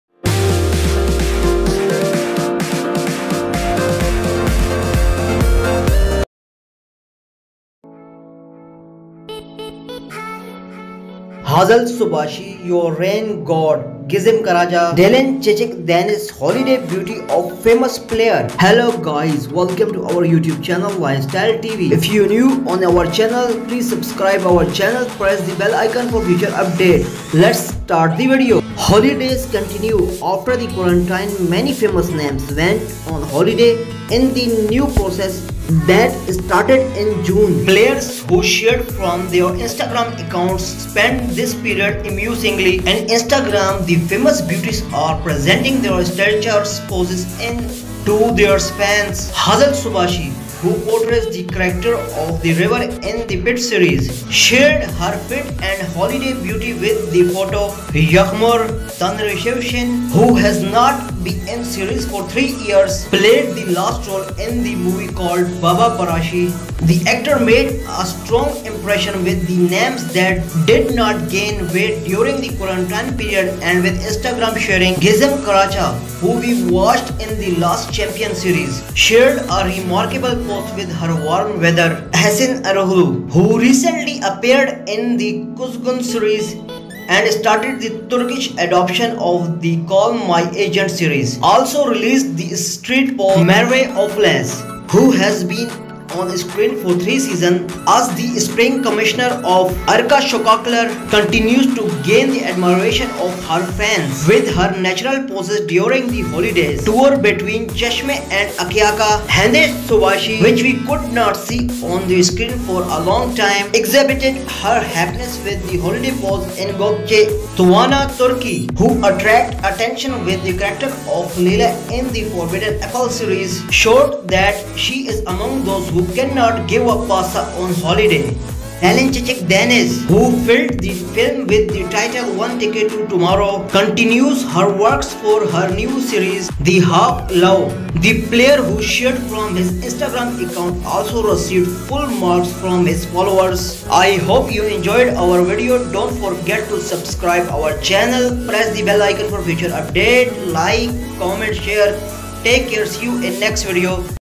Hazal Subaşı, You're Rain God, Gizem Karaca, Dilan Çiçek Deniz! Holiday Beauty Of Famous Players
Hazal Subaşı, Yağmur Tanrısın, Gizem Karaca, Dilan Çiçek Deniz!  Ünlü Oyuncuların Tatil Güzelliği

Turkish Translated